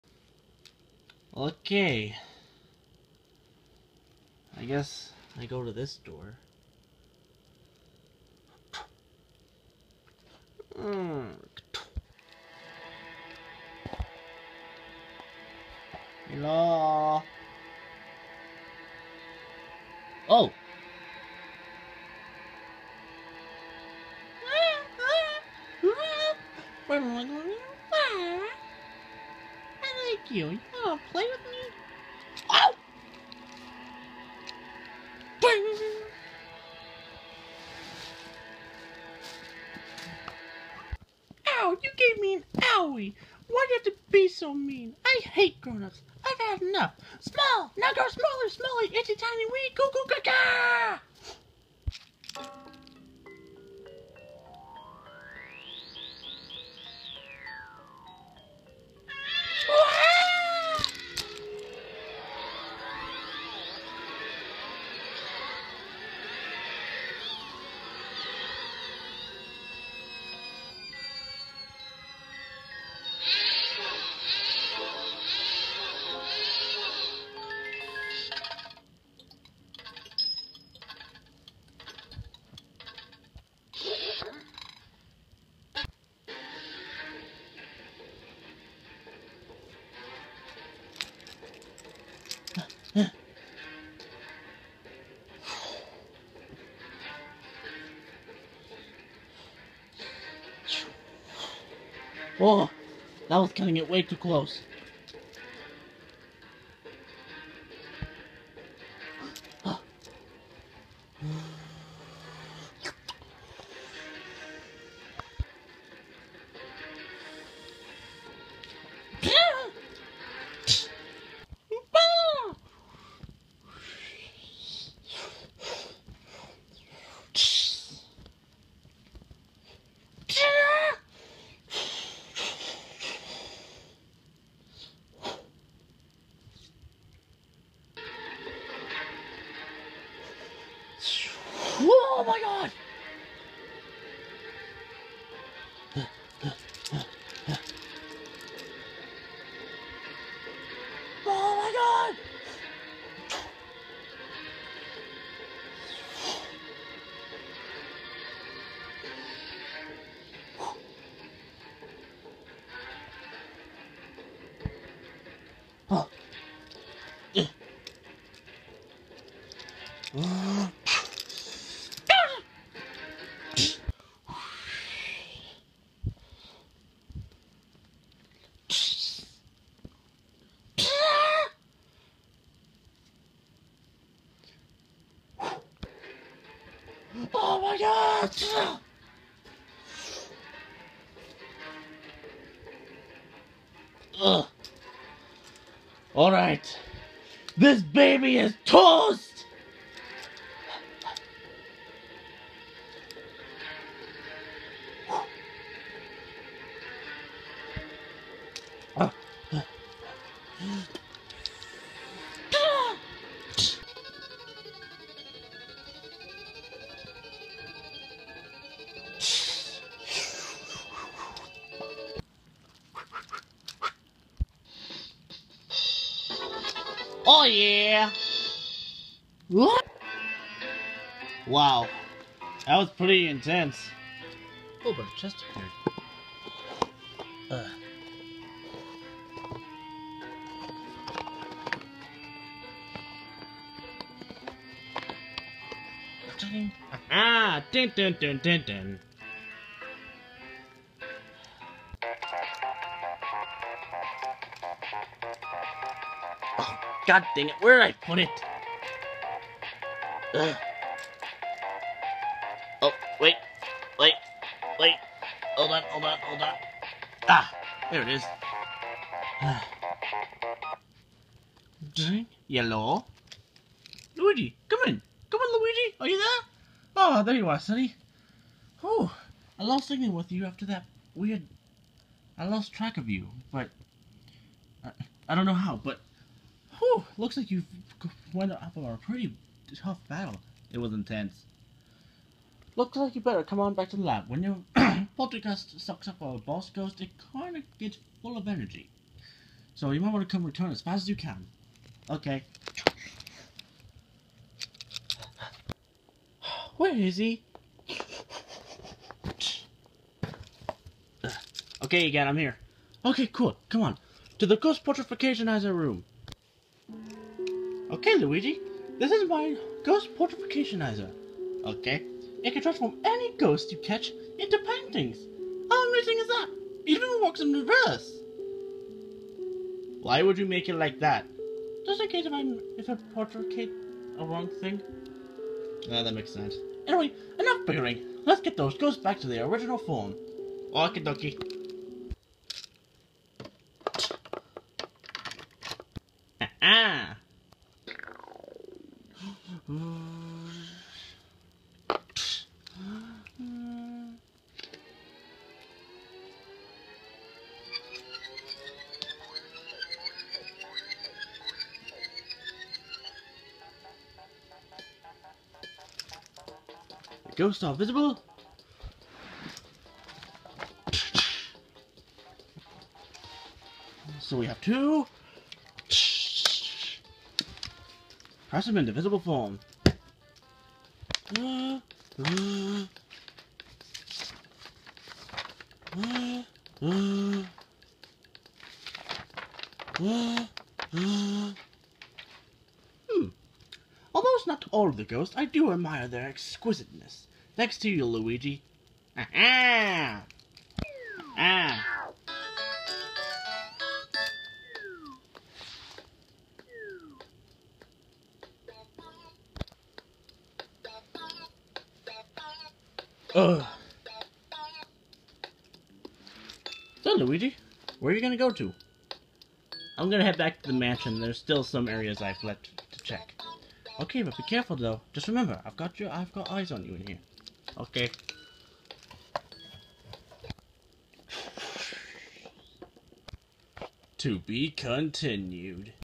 Luigi's Mansion Plush Ep. 6
Luigi finds a baby that's overpowered as heck.